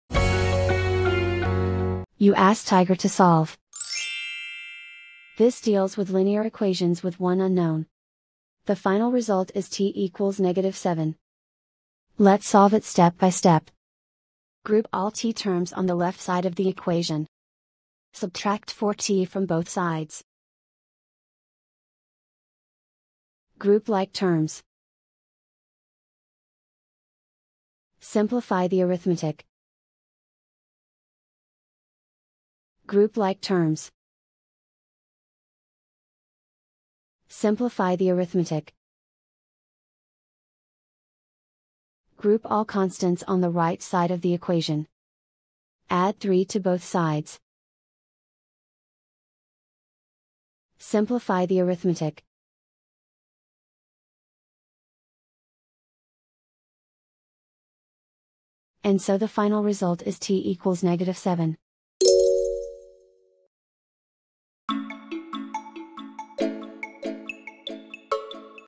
Solve 5t-3=4t-10: Linear Equation Video Solution | Tiger Algebra
Struggling with 5t-3=4t-10? Watch our step-by-step video on Tiger Algebra to master this linear equation effortlessly.